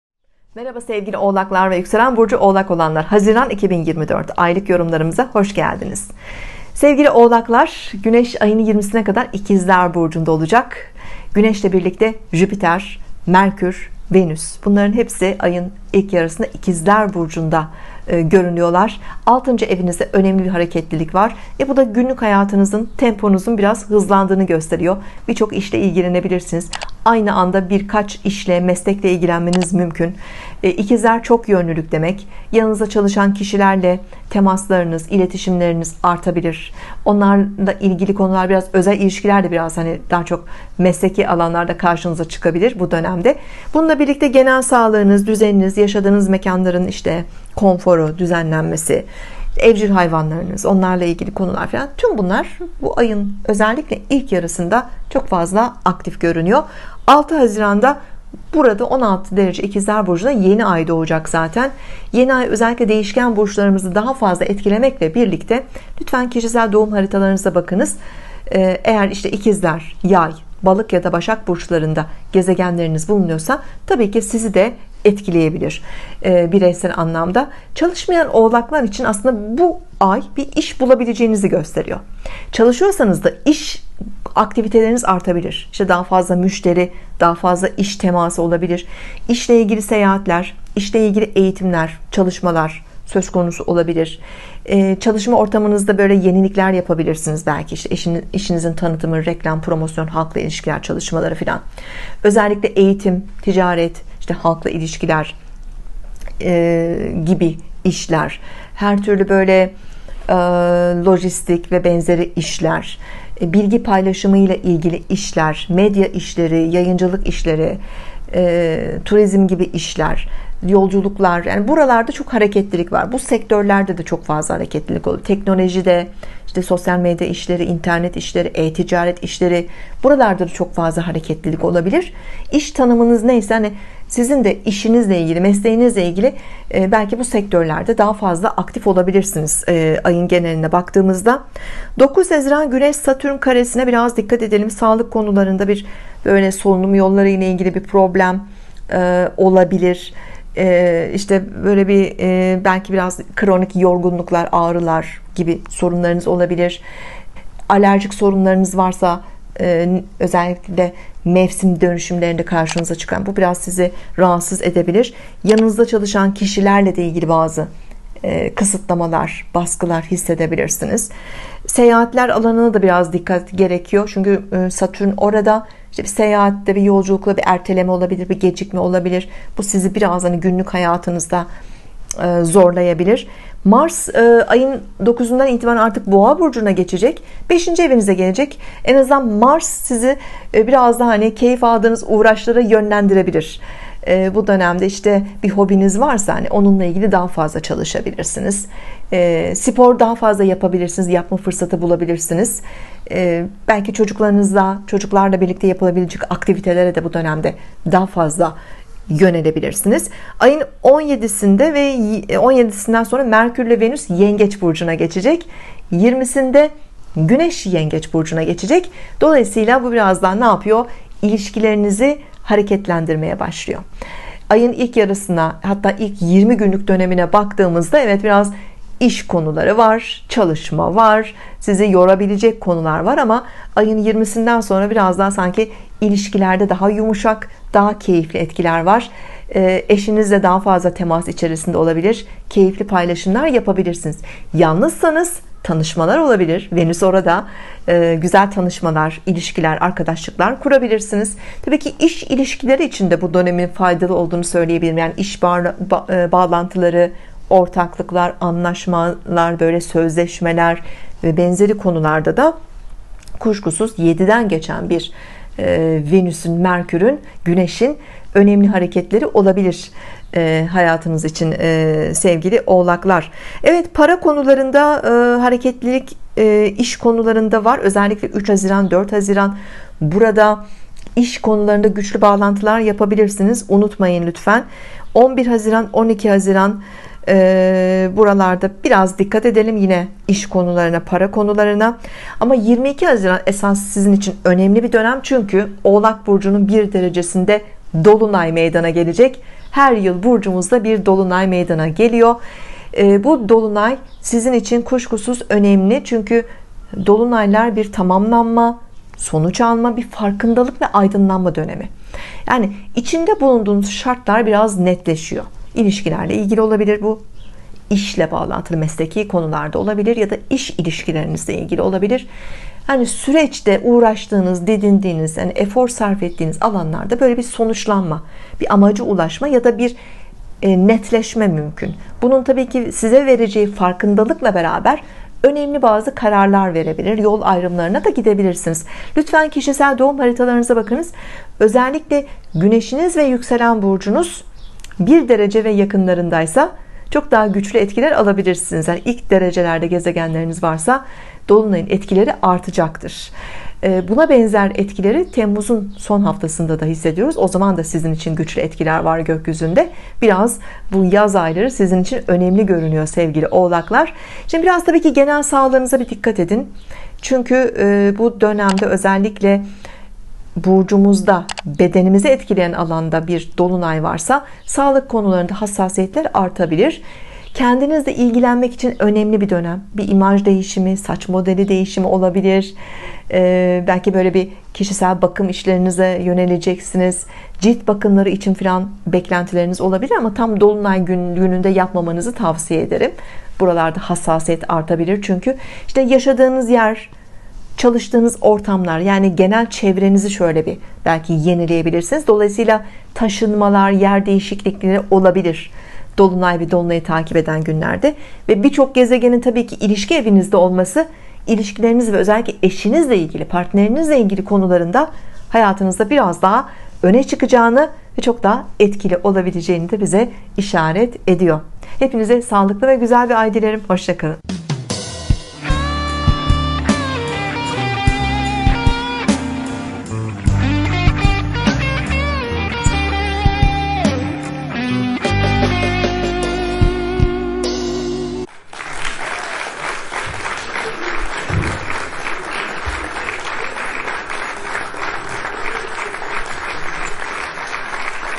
Oğlak Burcu, Haziran 2024, Aylık Burç Yorumları, Astrolog Demet Baltacı ile Astroloji haber burçlar
00:00 OĞLAK Burcu Haziran 2024 yorumu
01:52 Yeniay ve etkileri
02:20 Yeniay en çok kimleri etkileyecek
05:12 Dolunay ve genel etkileri
06:04 Dolunaydan kim daha çok etki alacak

07:35 Mayıs size neler getirecek ?
10:00 Bitiş